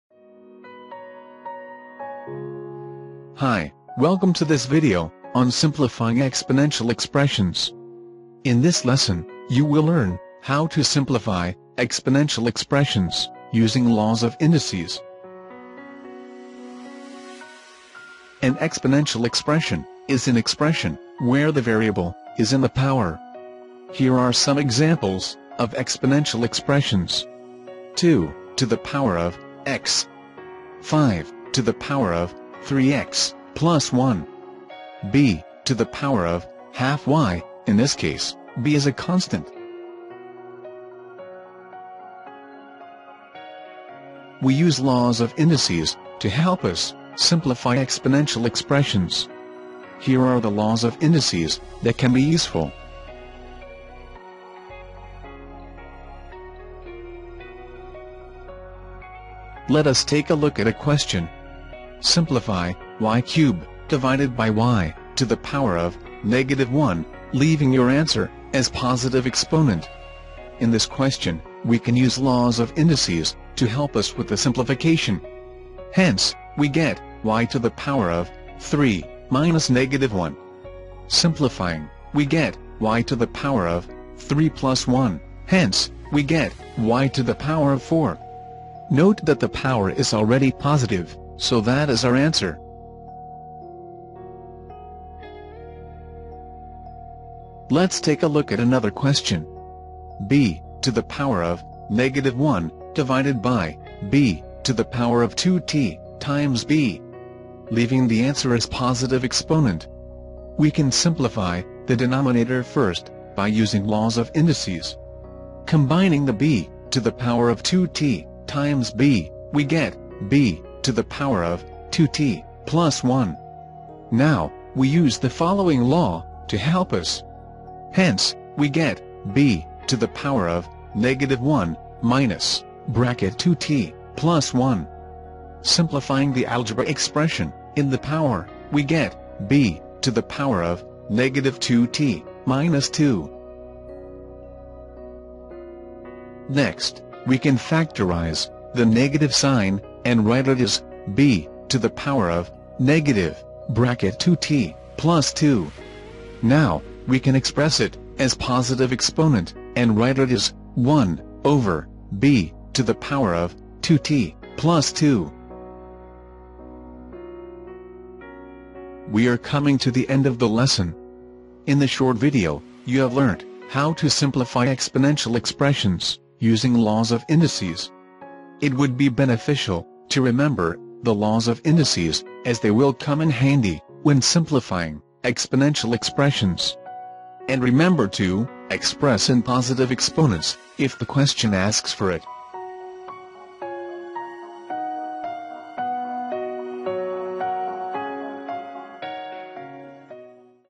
Exponential Expressions
This video lesson illustrates the concept on the simplification of exponential expressions.